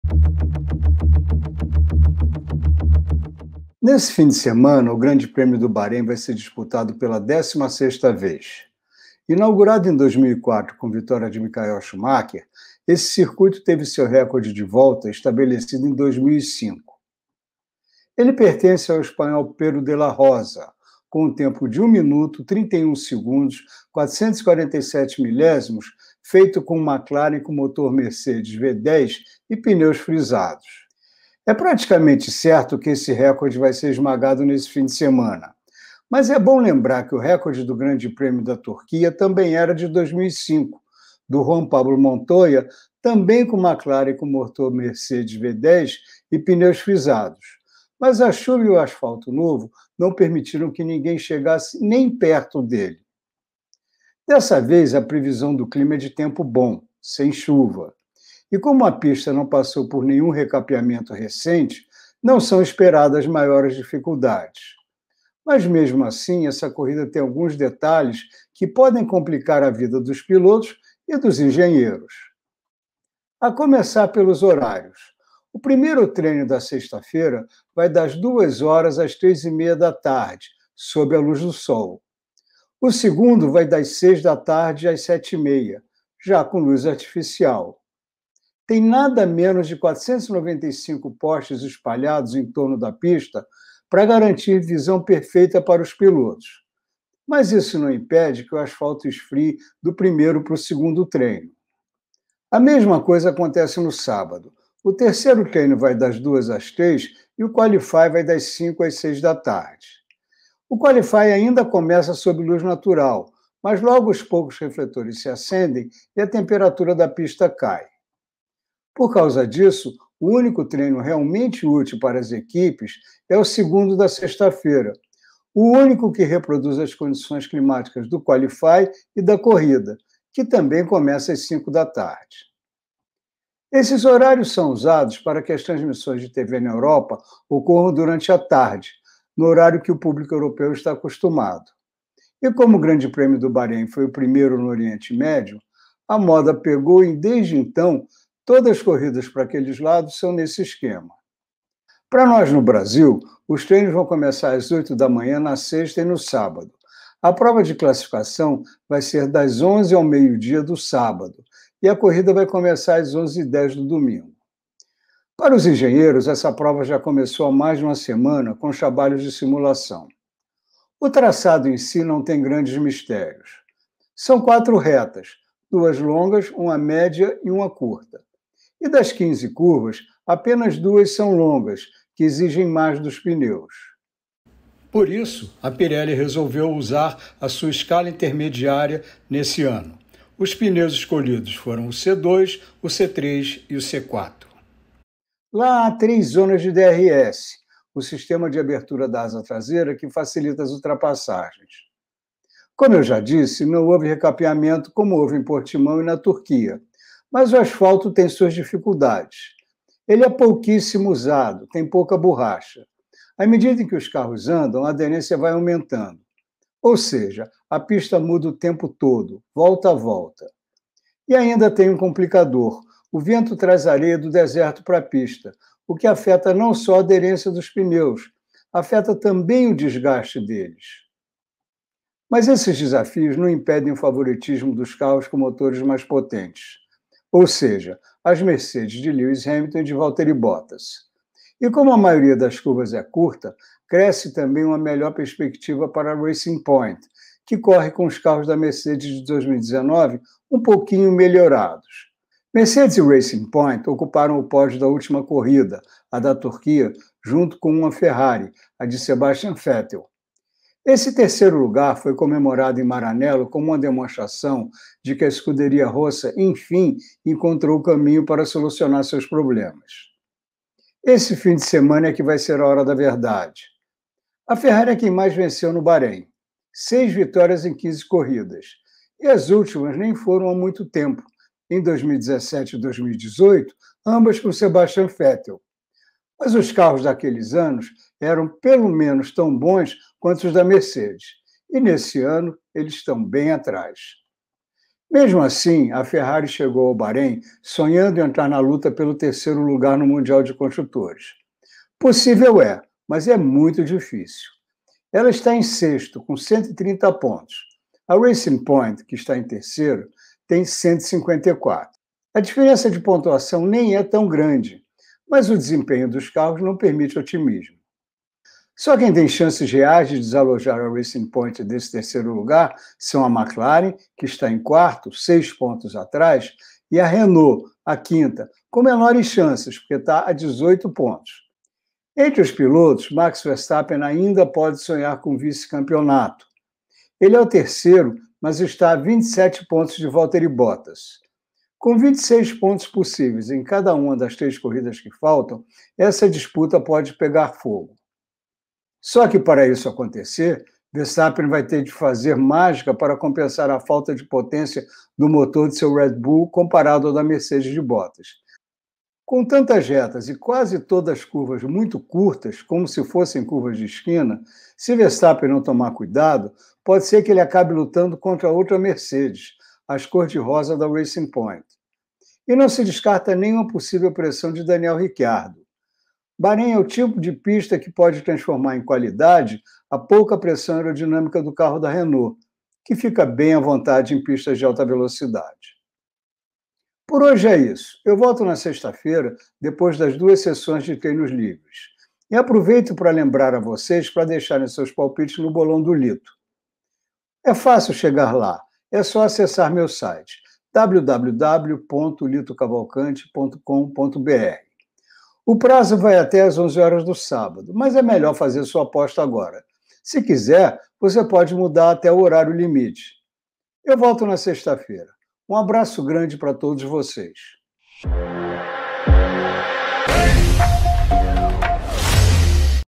GP DO BAHREIN: VERSTAPPEN AINDA QUER TIRAR O VICE-CAMPEONATO DE BOTTAS
A 27 pontos do finlandês, o piloto da Red Bull ainda tem chances de chegar ao vice-campeonato, que seria sua melhor colocação na F1. E mesmo que o circuito  barenita, com quatro retas e curvas curtas, seja  mais favorável à potência dos motores Mercedes do que à agilidade do carro da Red Bull, o holandês promete lutar até o fim. Principalmente porque já não tem mais nada a perder.